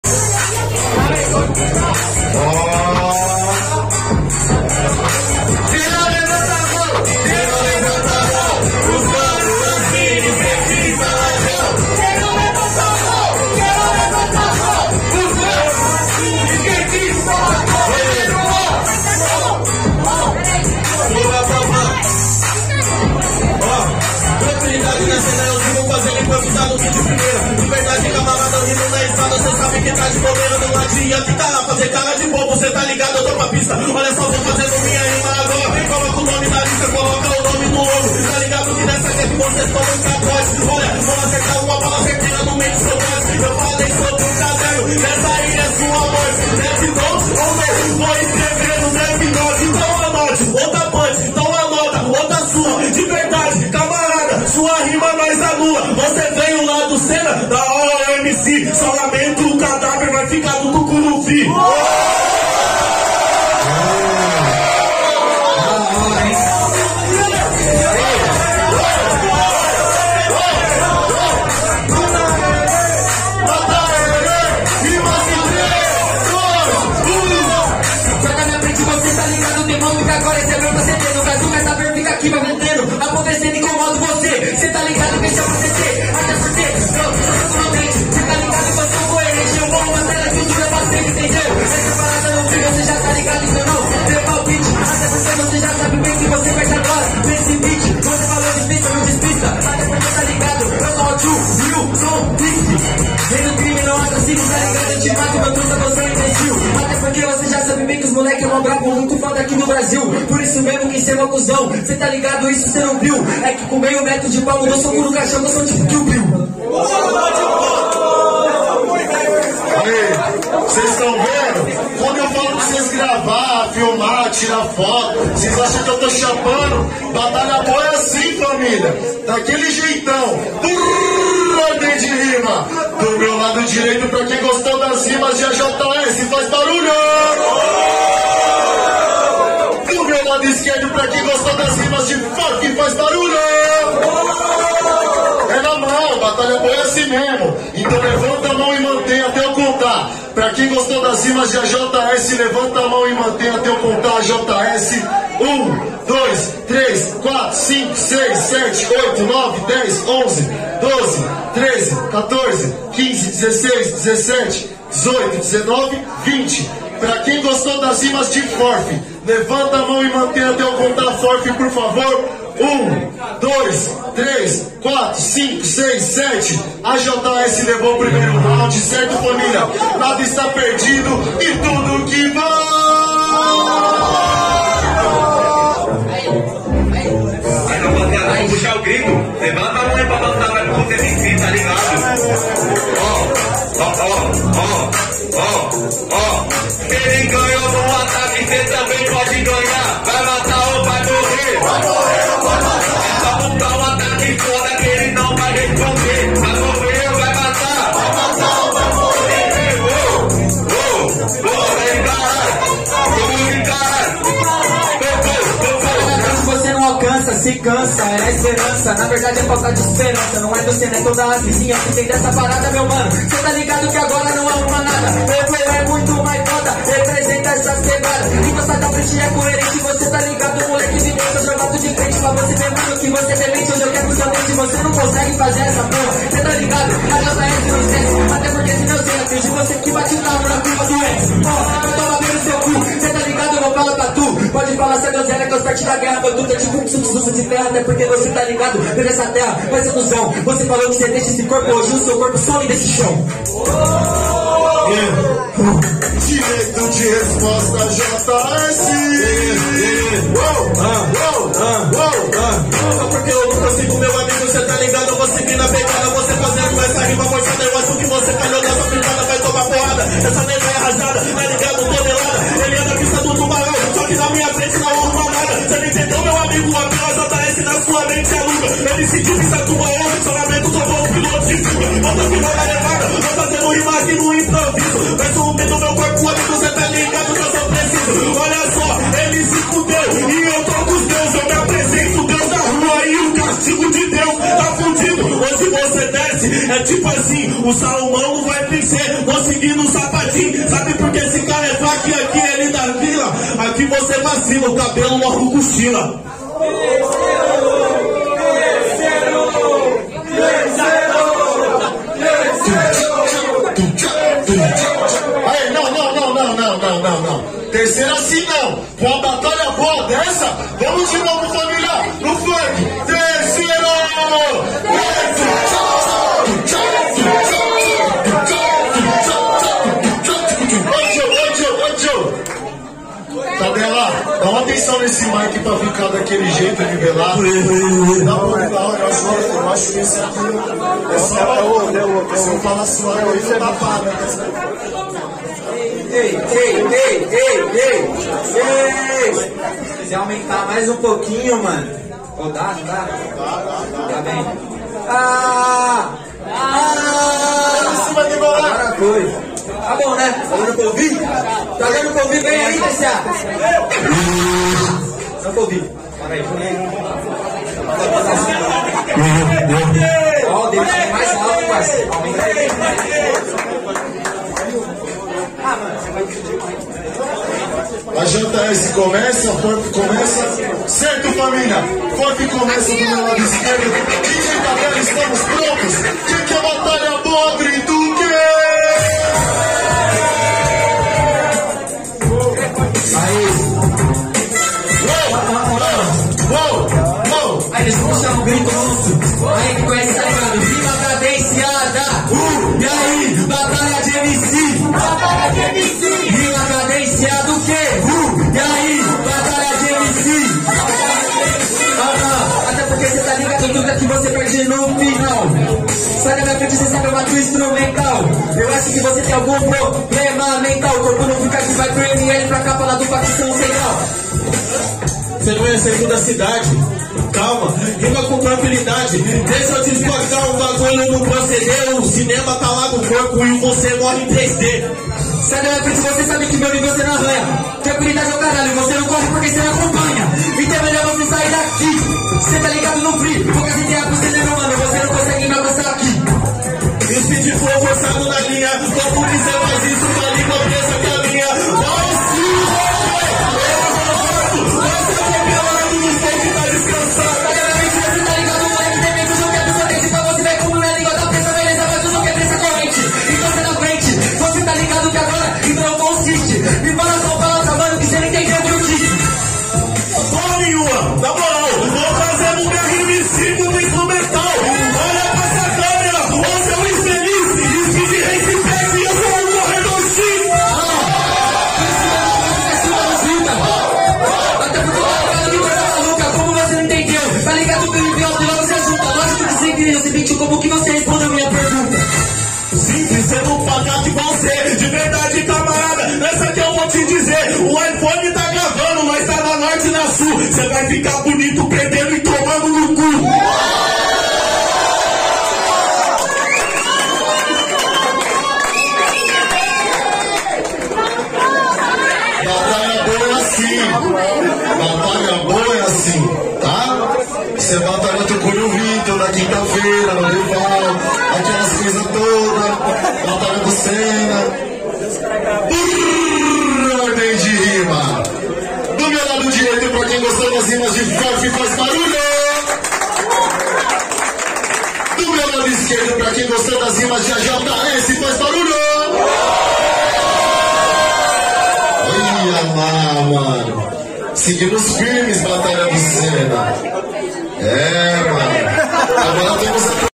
(O MELHOR FLOW DA NOITE) AJS x Forfe | BATALHA TUCURUVI vs BATALHA DO SENNA | 22ª
ENDEREÇO: AVENIDA LUÍS DUMONT VILARES, 1617 - SÃO PAULO - SP
TODA QUINTA-FEIRA A PARTIR DAS 18:00 ATÉ AS 23:00